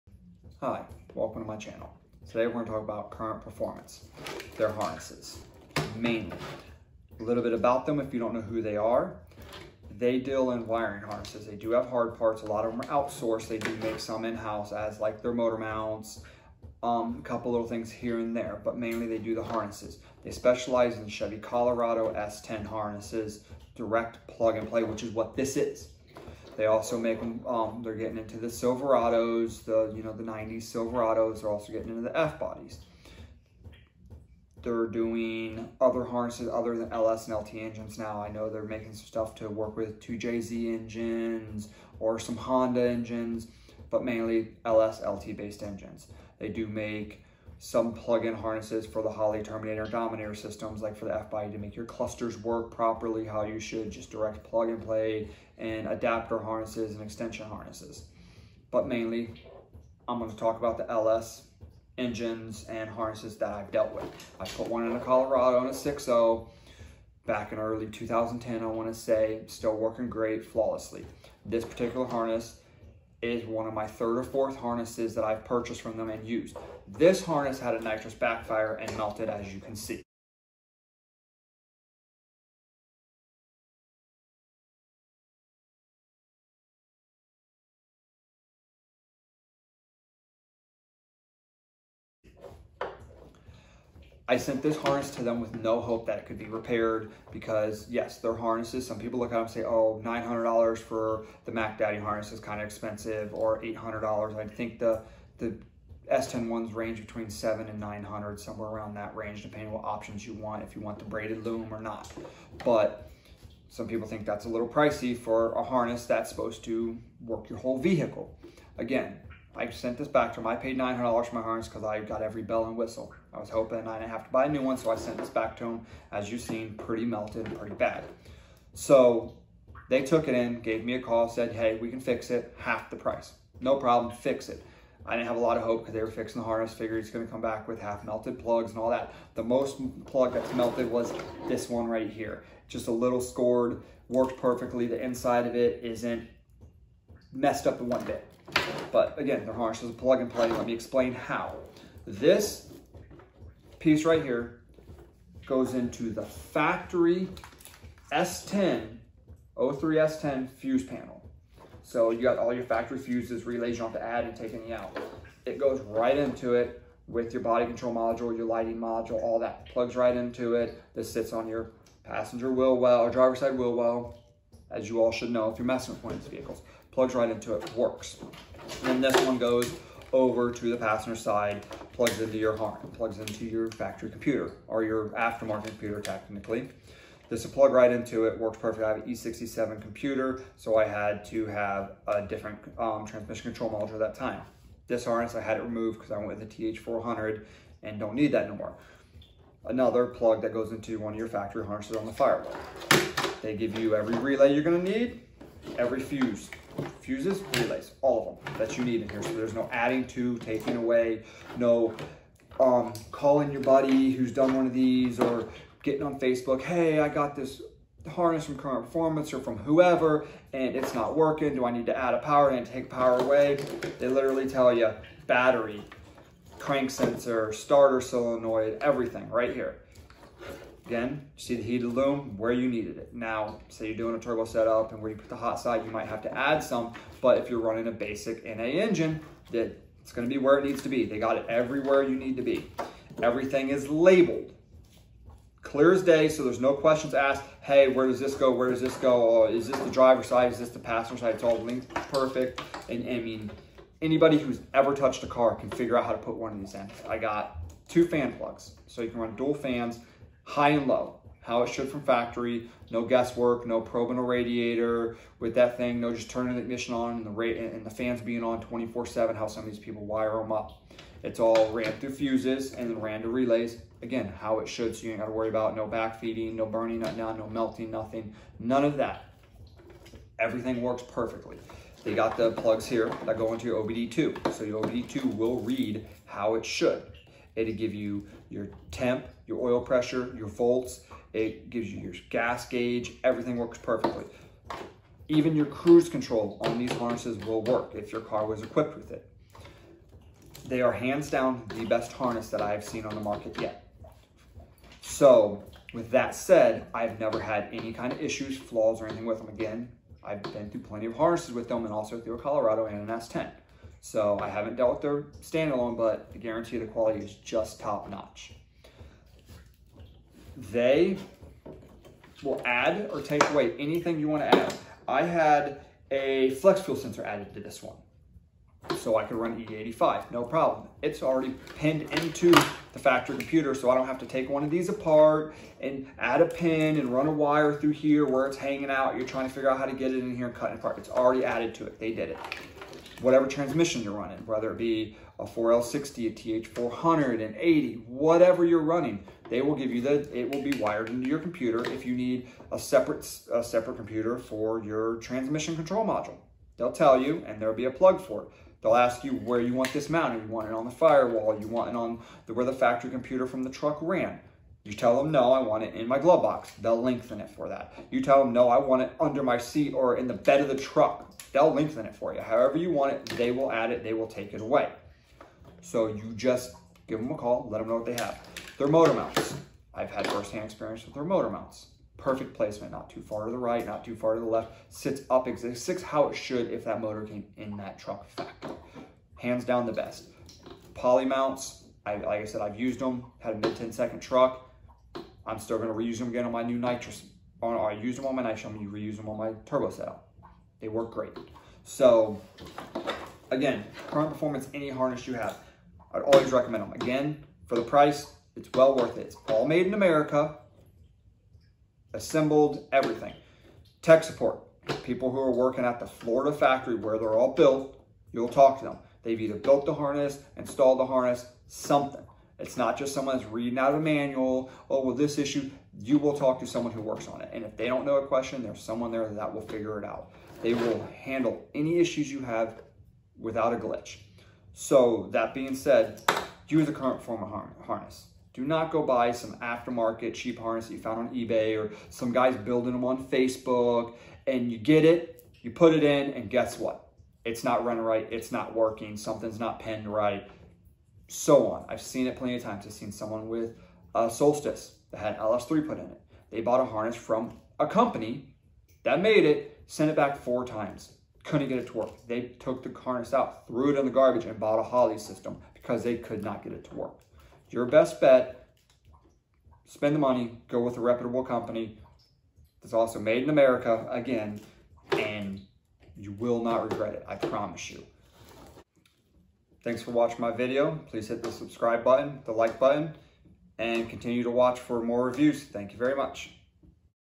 Current Performance wiring harness review
This is my personal review of the current performance wiring harness review and real world experience with them. This harness was for a LS s10 swap